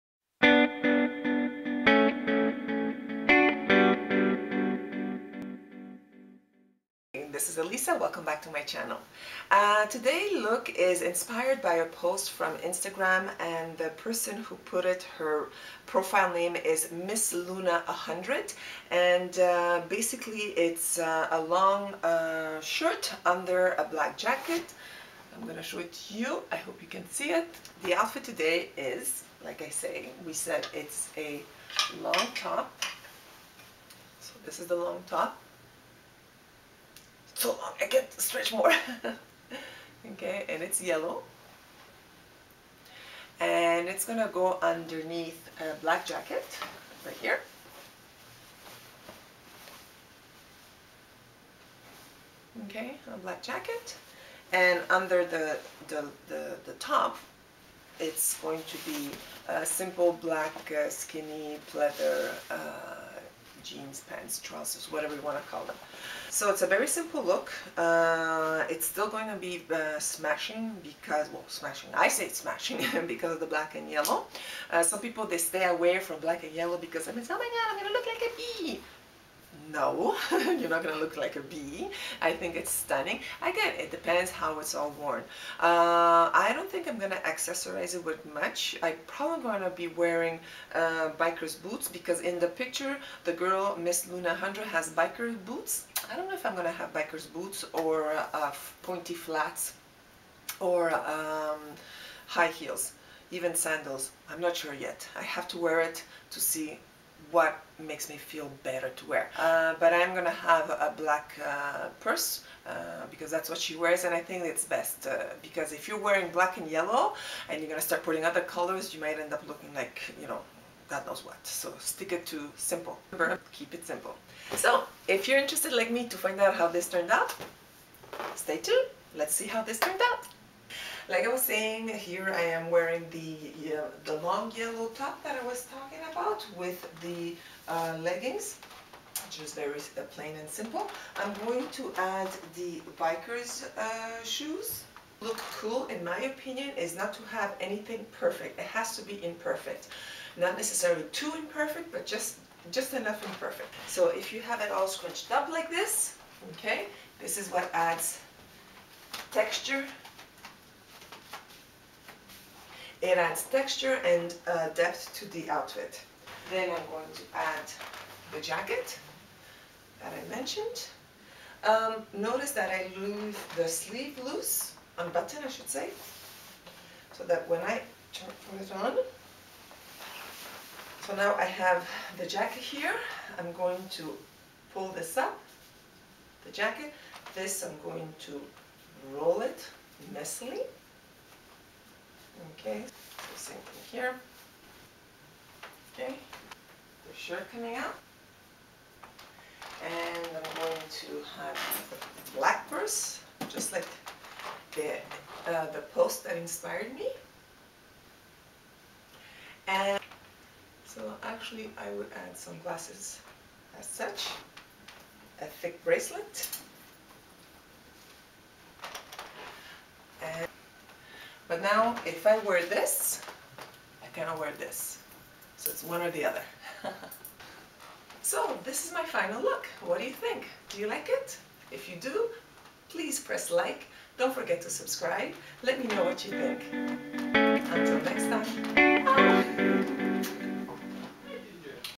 Yellow dress over trousers (in my 50s)
Today's outfit or OOTD:

HM black jacket
Stitches treated black skinny jeans
Zara puce tunic
Urban Planet earrings
Forever XI bracelet & choker
Forever XI booties
Dooney & Burke purse
Ralph Lauren sunglasses